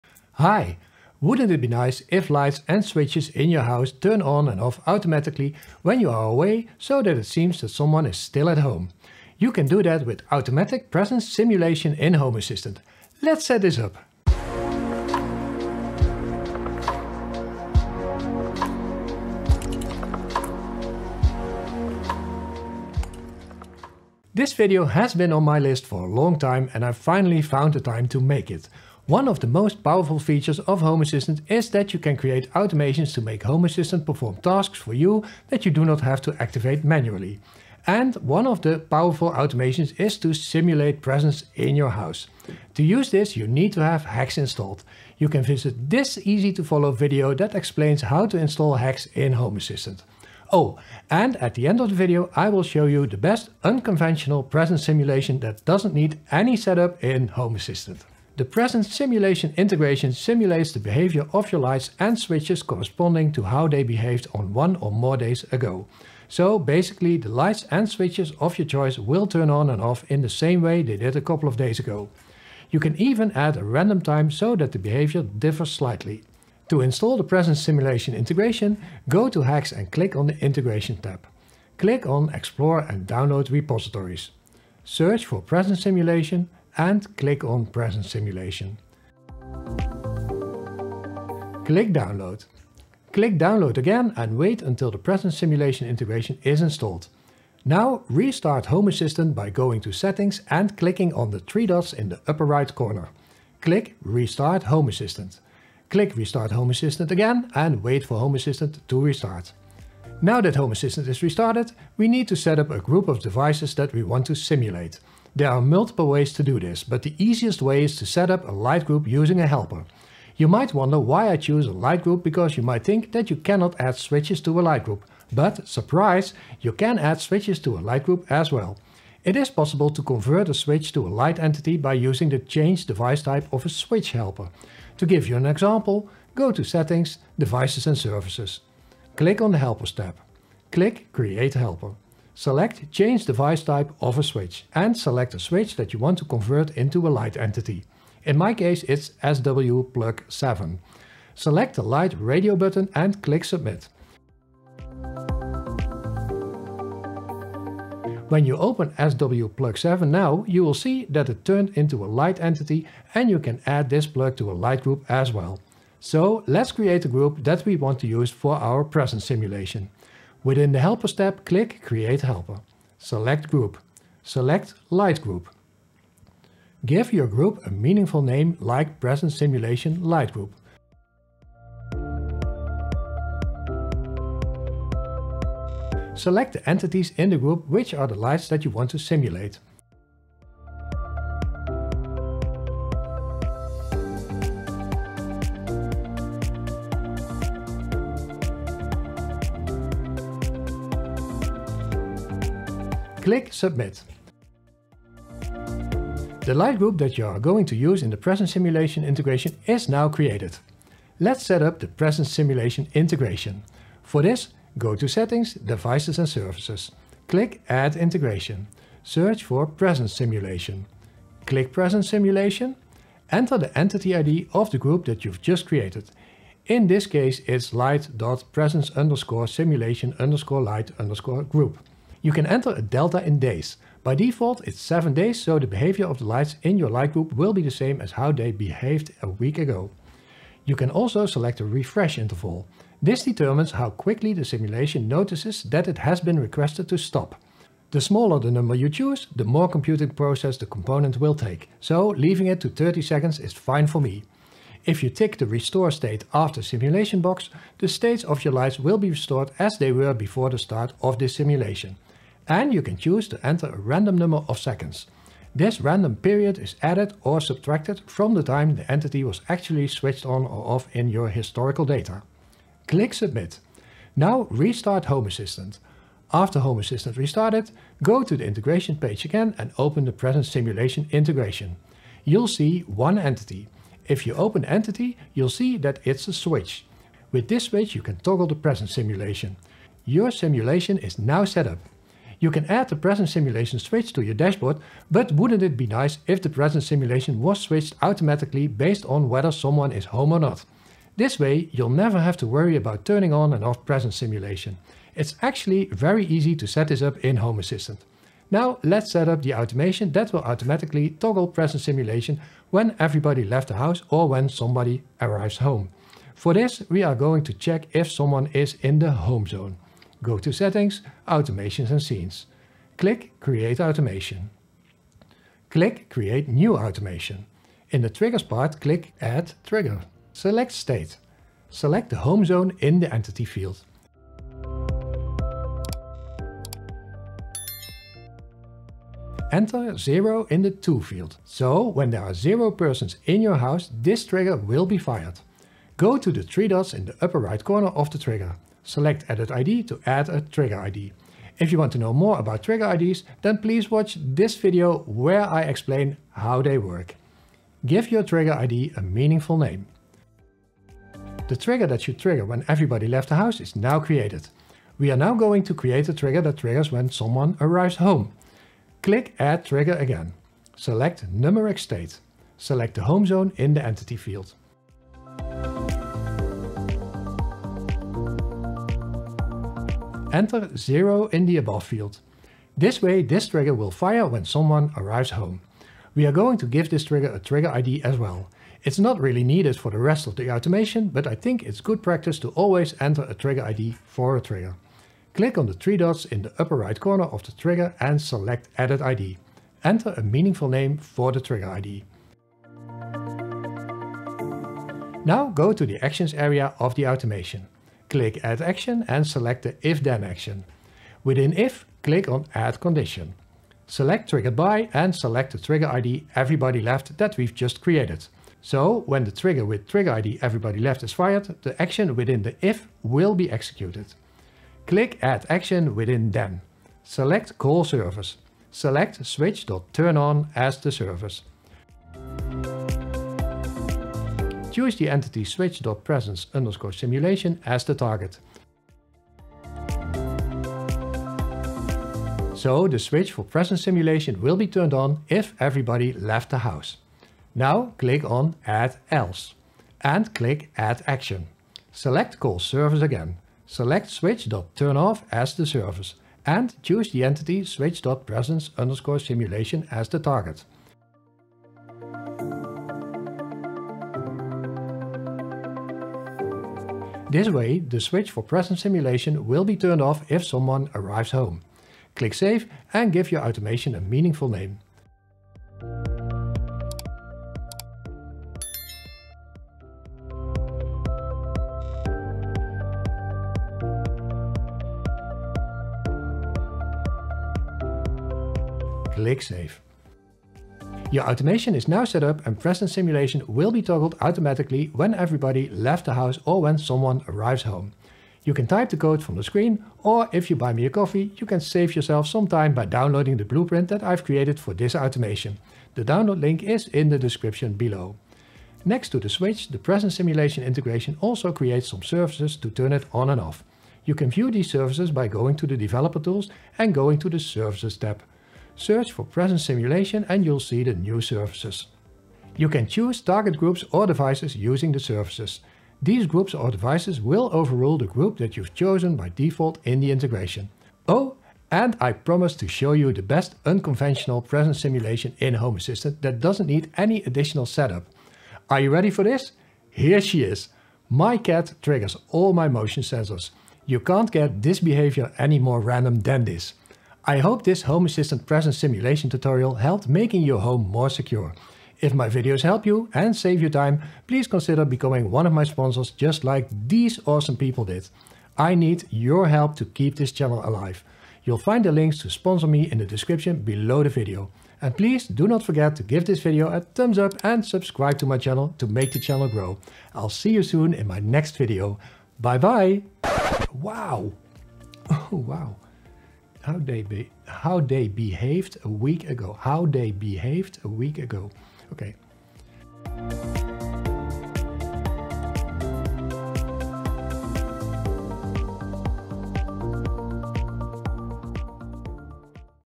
Automatic Presence Simulation in Home Assistant - Tutorial
This Home Assistant Tutorial explains how you can set up Presence Simulation in Home Assistant so that it seems that someone is at home while you are away. I will explain how you can set up simulating presence in your house and how to set it up so that it will be activated automatically as soon as you leave your house. This advanced automation will help to secure your home.

⭐ You can download the code that I created for this video

⭐ I offer Consultancy and Training for your Home Assistant setup.

00:00:00 Introduction
00:00:58 How to Install the Presence Simulation Integration
00:01:50 Setting up a group of devices to simulate
00:03:30 How to Set up the Presence Simulation Integration
00:04:52 Toggle Presence Simulation Automatically based on presence.
00:05:08 Setting up the Automation
00:08:18 Downloading the Blueprint
00:08:50 The best presence simulation!